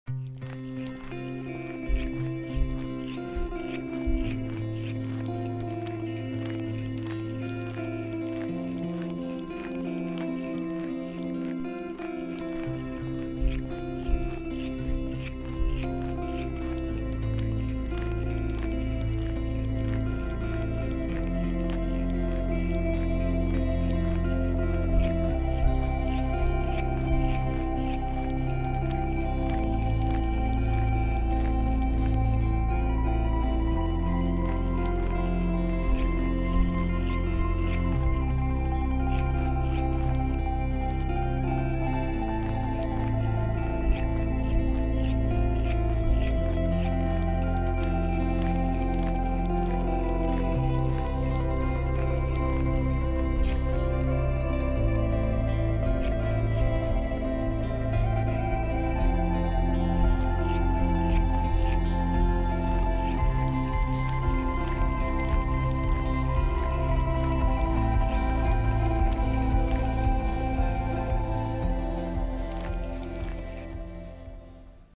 Kitty Washing Up
Peaches washing herself.